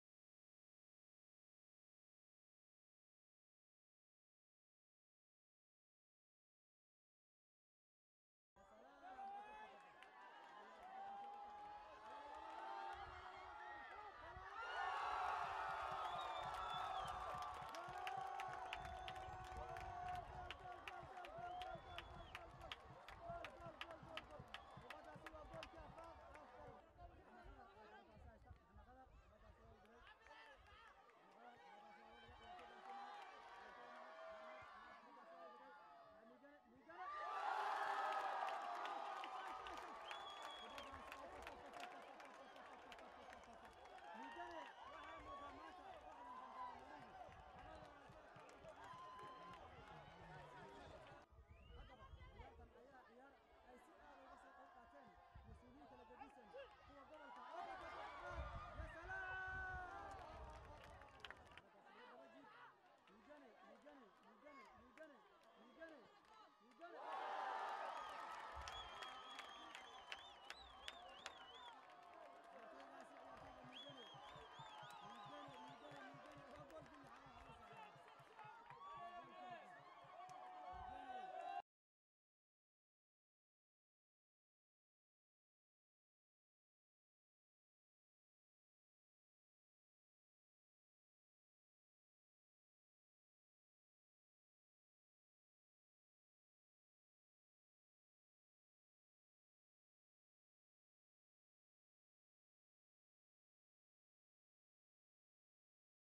SEJS/DJIBOUTI/ TOUTES LE BUTES finale de la super coupe à Tadjourah
Un match tant attendu par les spectateurs passionné du ballon ronde //
la combativité sur le terrain, des contres  monstrueuses de deux équipes  et surtout le désir ardent de gagner fait que ce match reste inoubliable et marqué a tout jamais dans l’histoire du football Djiboutiens. //
le numéro 13 et 21 de la gendarmerie sont des joueurs galactiques et à  la fois l’espoir pour les spectateurs qui sont venu en masse et qui les obligeaient à donner le meilleur de eux même //
La vitesse du jeu et les prouesses qu’il autorise, l’enthousiasme que suscite un match, des moments des émotions fortes, même hostile entre équipes adverses tout cela se traduisaient dans un rythme plus ou moins fort par les deux équipes rivaux qui n’avaient de cesse d’éveiller en nous cette passion que nous nourrissons pour les ballons rond . C’est un vrai régal pour les amoureux du foot et c’est ce qui donne une bonne raison de croire que la vie vaut la peine d’être vécu. //